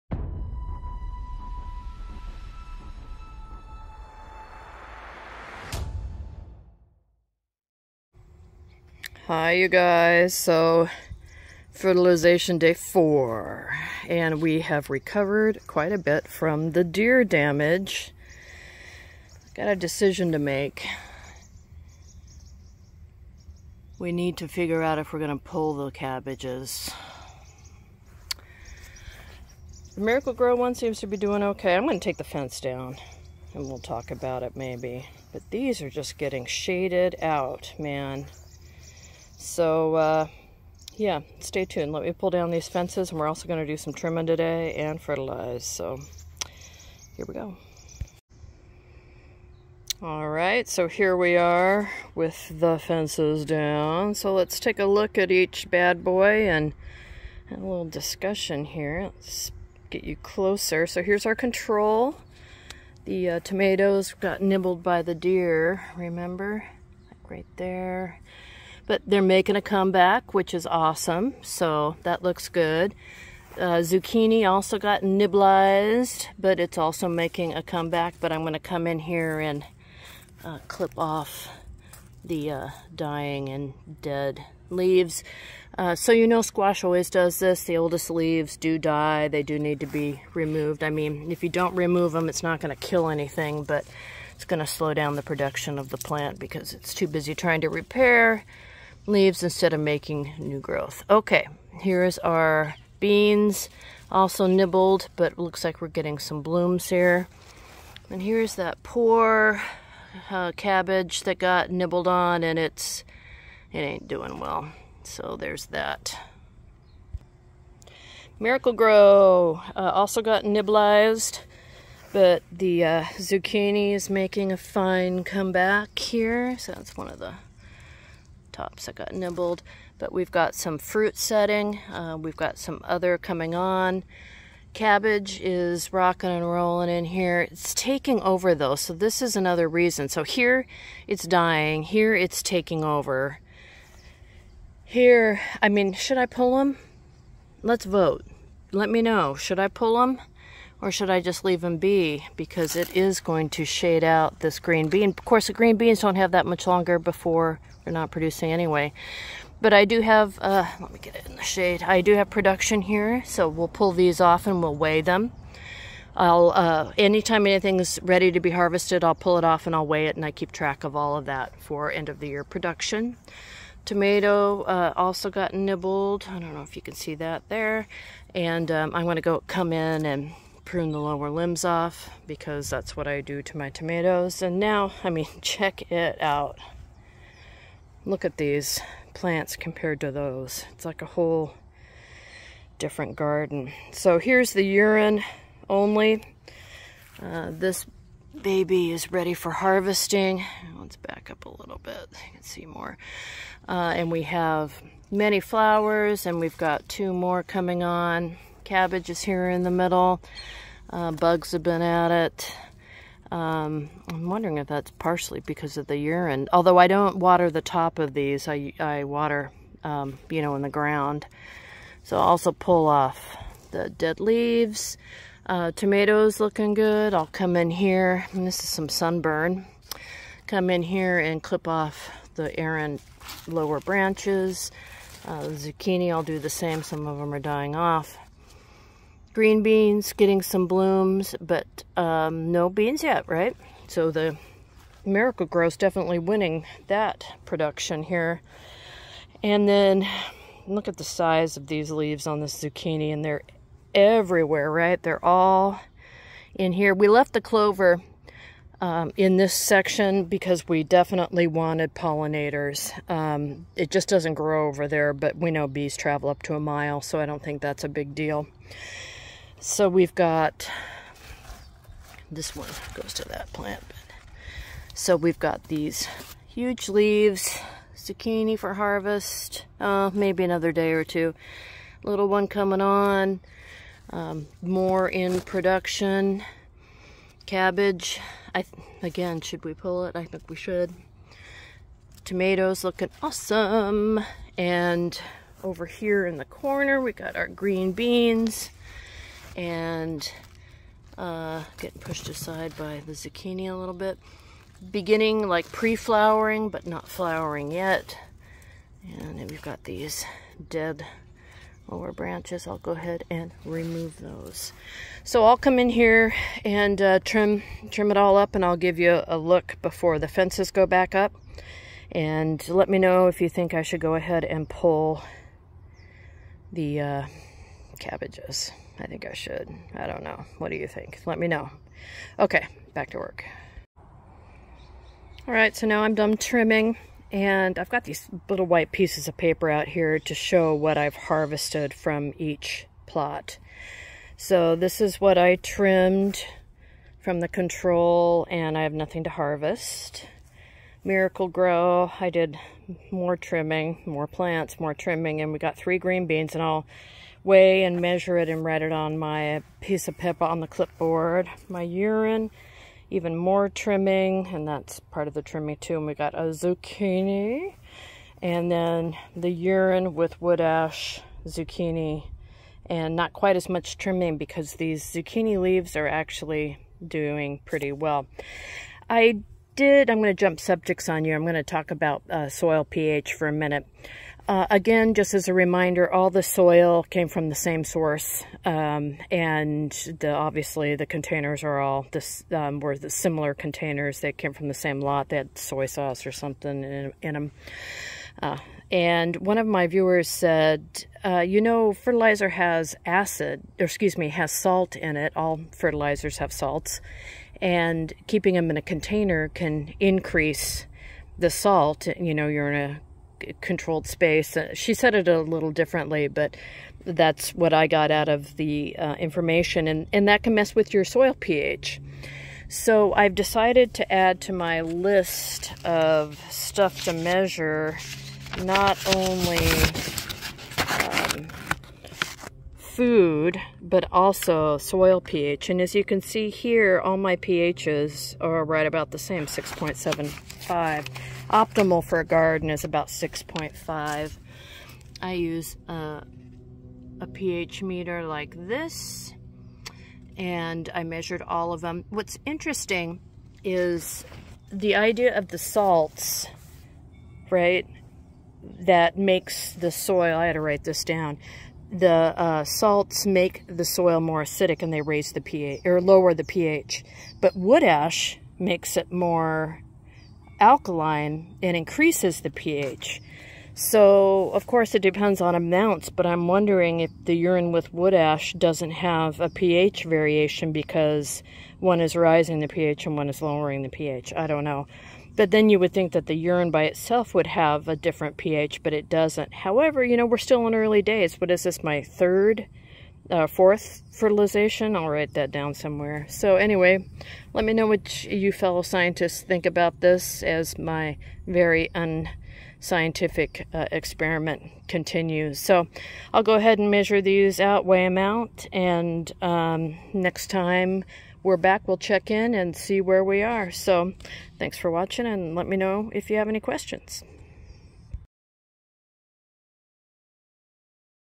Miracle Grow V. Urine and Urine with Wood Ash Video #4
Video #4 after several applications of Fertilizer(s)
...We are year 4 into testing how good Urine is as a fertilizer...again we test against Miracle Grow and Urine mixed with Wood Ash.  This year we are testing:
Black Cherokee Tomato, Cabbage, Zucchini and green beans (from seed).  Let's get started!